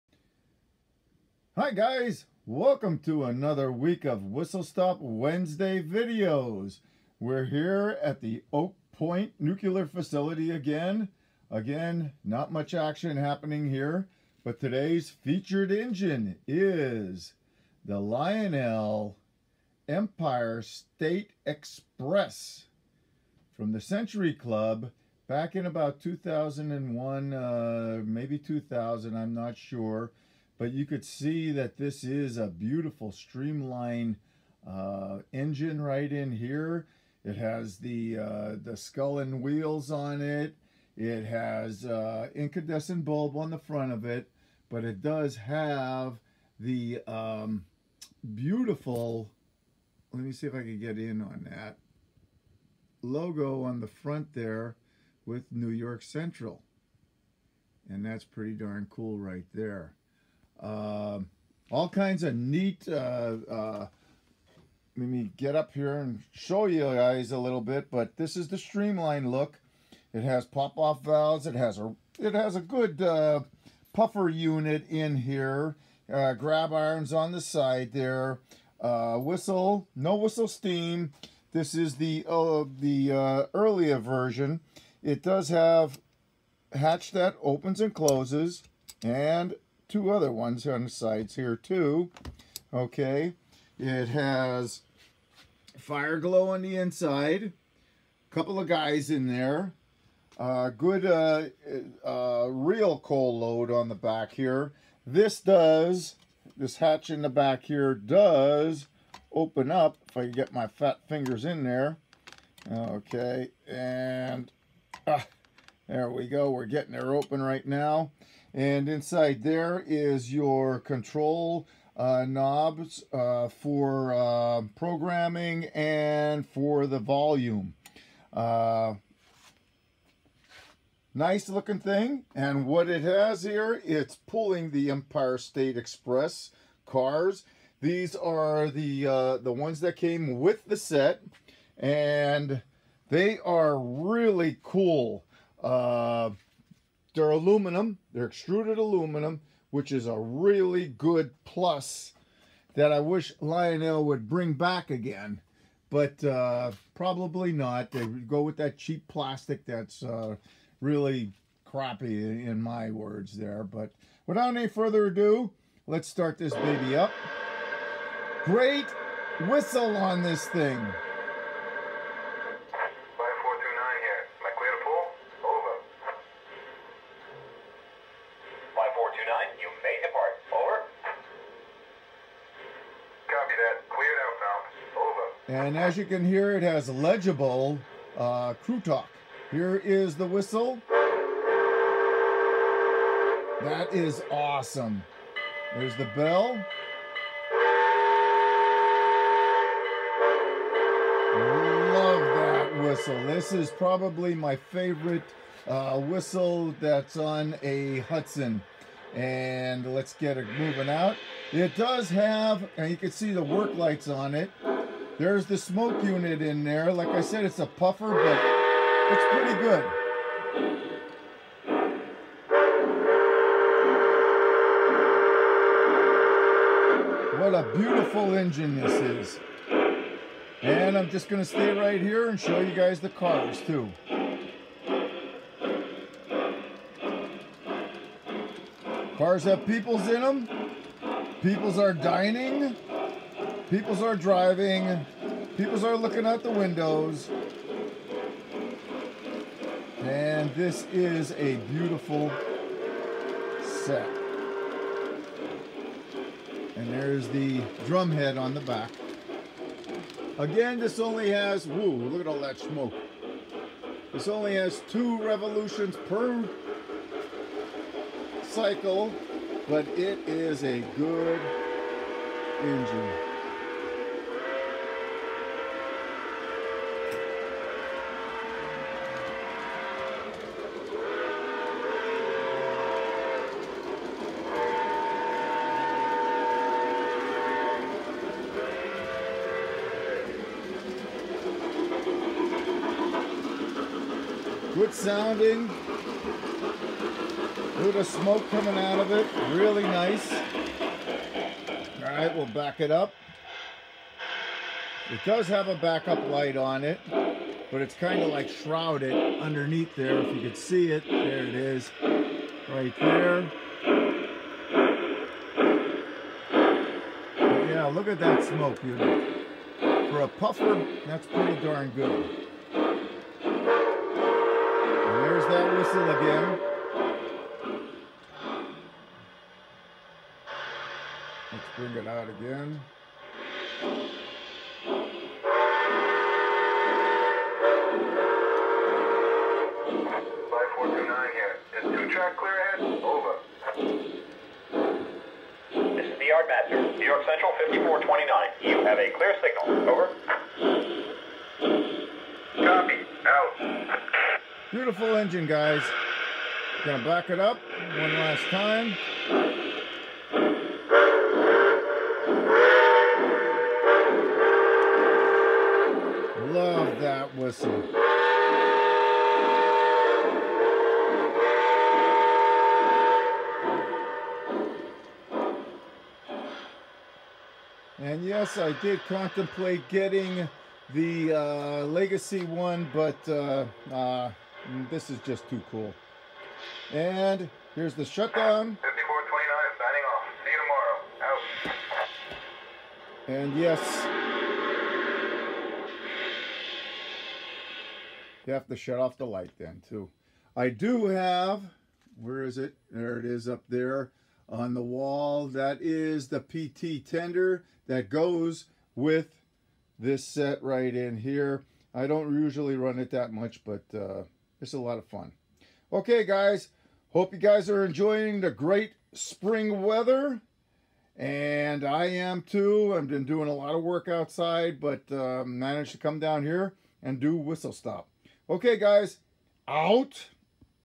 Whistle Stop Wednesday Videos 03/24/21
Welcome to Whistle Stop Wednesday Videos.  This weeks featured engine is the Lionel TMCC CC Empire State Express set., from early 2000s.  A Beautiful stream line engine along with extruded aluminum passenger cars.  One of my favorite sets, hope you enjoy this video.